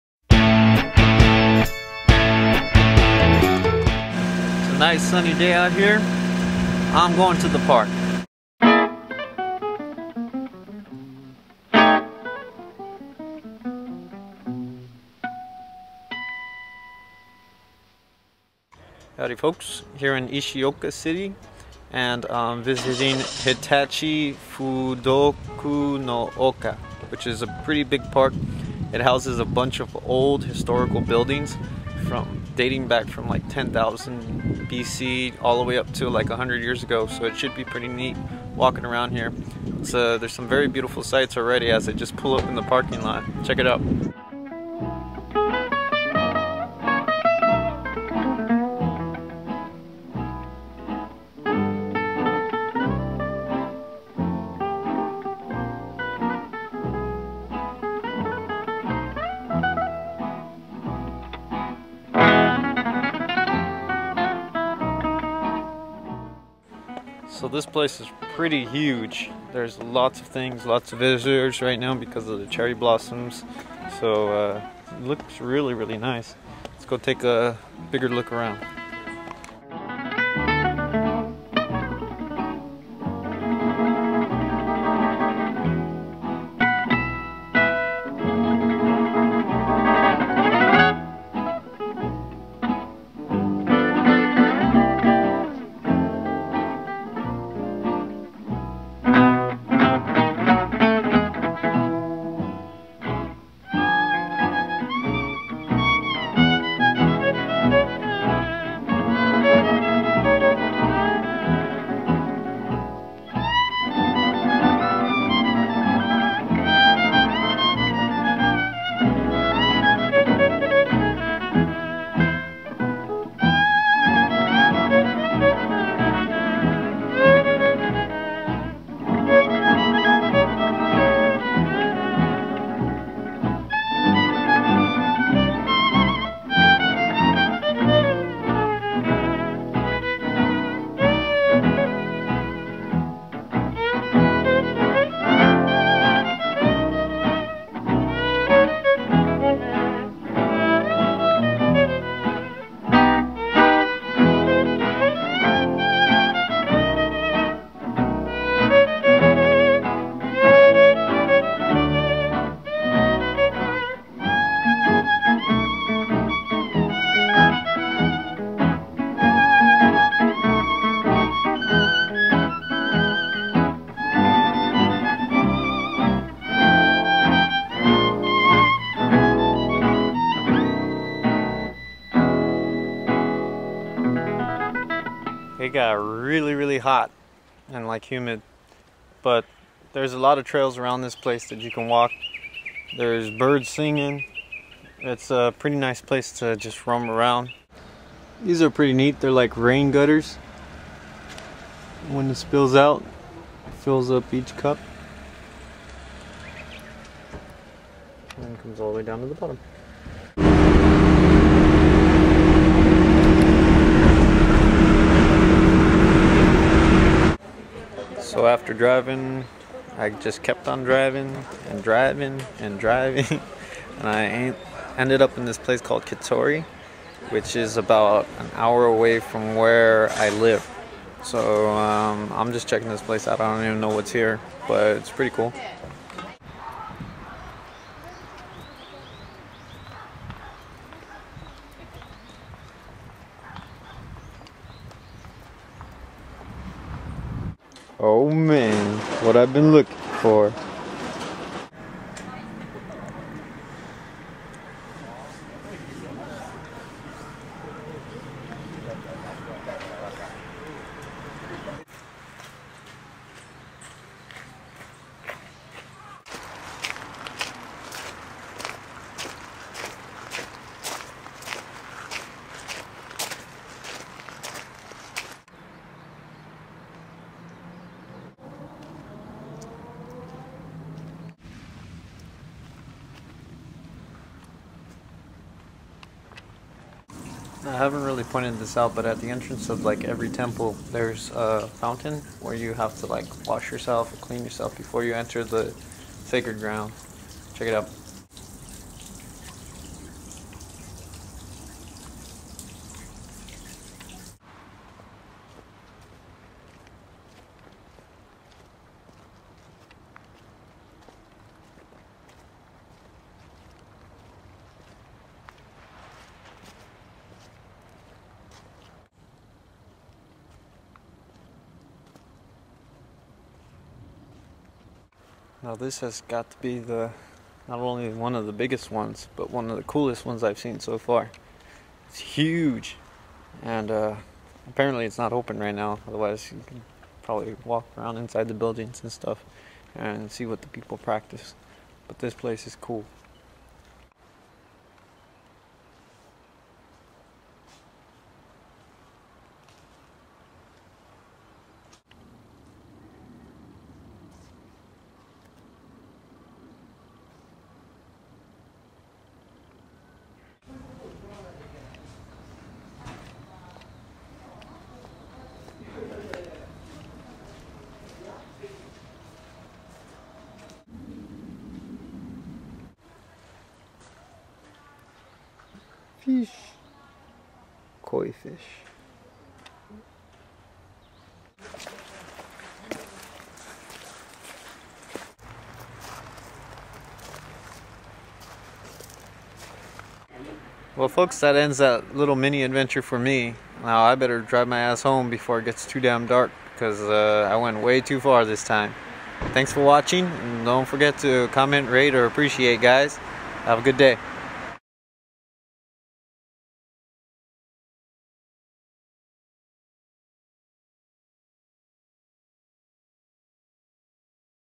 Markie in Japan - Day in the Park and Katori
It was a nice day so I made the most of it. I drove pretty far and ended up in Katori. Its in the Chiba prefecture. I have many photos of this and other events on my flickr.